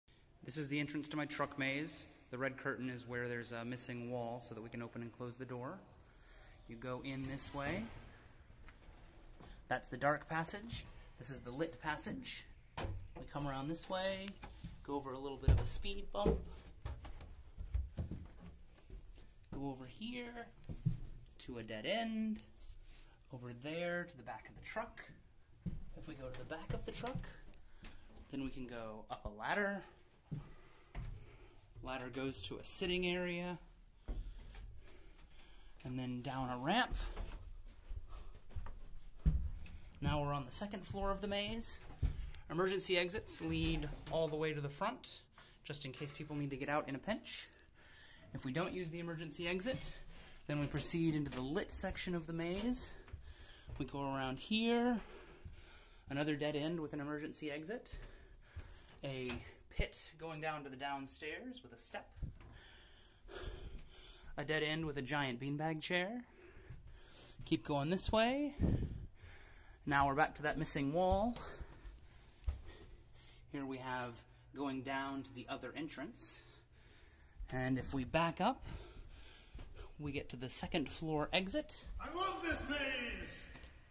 Truck Maze tour
A walkthrough of the maze I built in the back of a moving truck for a carnival-like event.